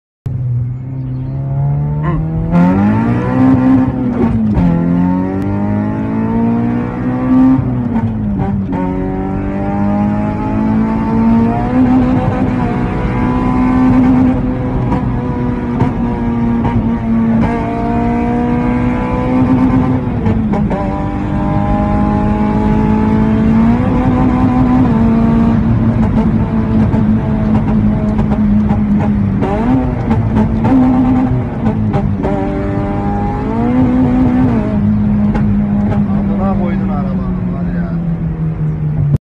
How to play music by pushing the gas pedal!!!
تخته گاز
شیطونی با پژو